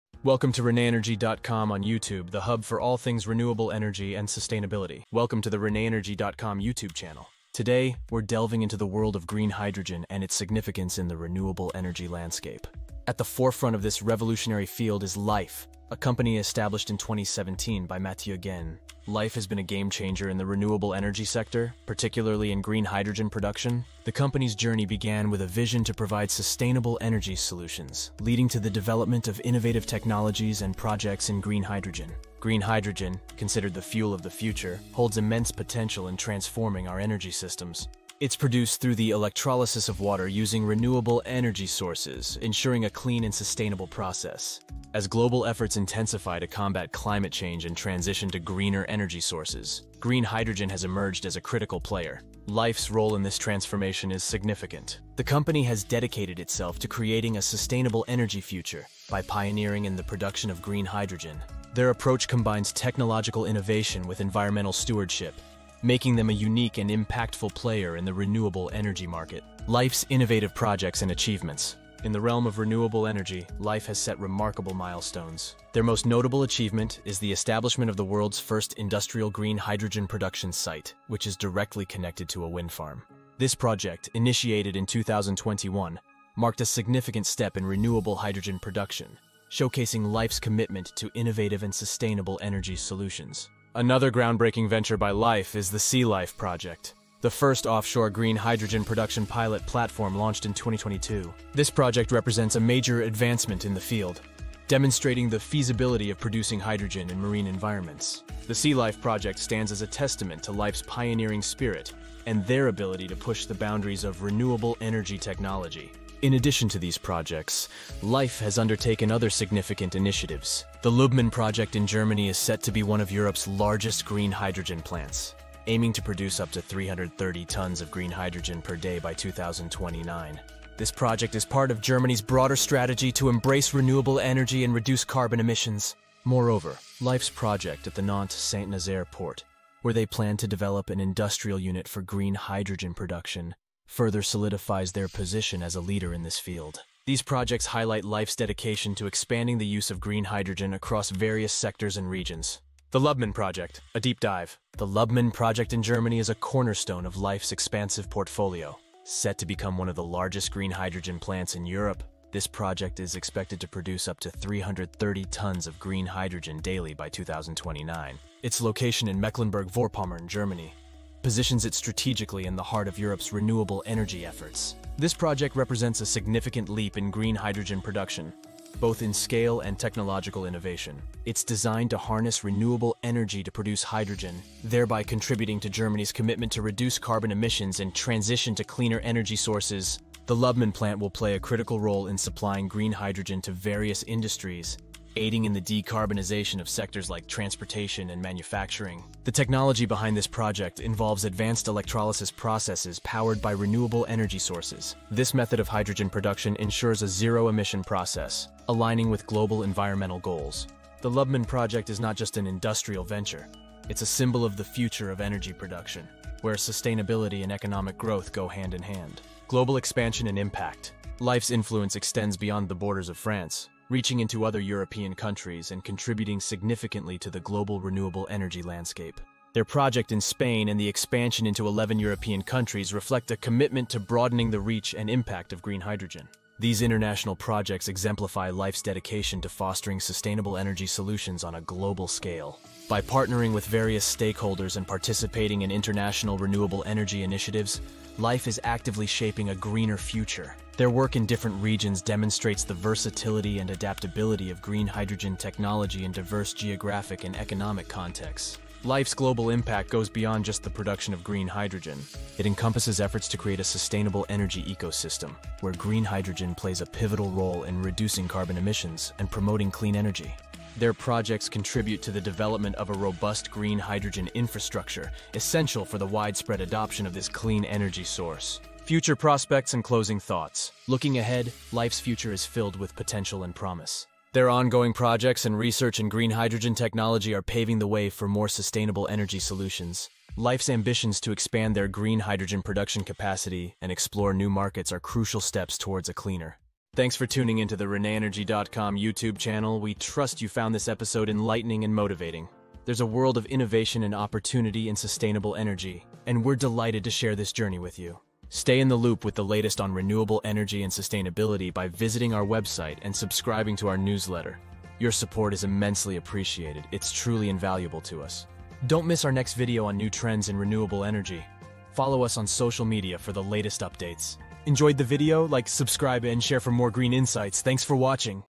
Lhyfe's Green Hydrogen Wave - Powering the Future
Dive into the world of renewable energy with "Lhyfe's Green Hydrogen Wave: Powering the Future".

In this enlightening video, we explore the innovative strides made by Lhyfe, a trailblazer in green hydrogen technology. From harnessing wind power to pioneering the world's first offshore green hydrogen production, Lhyfe is reshaping our energy landscape.

Join us as we delve into their groundbreaking projects, including the massive Lubmin plant in Germany and their expansion across Europe. Discover how Lhyfe's vision is setting new standards in sustainability and driving the global transition to cleaner energy sources.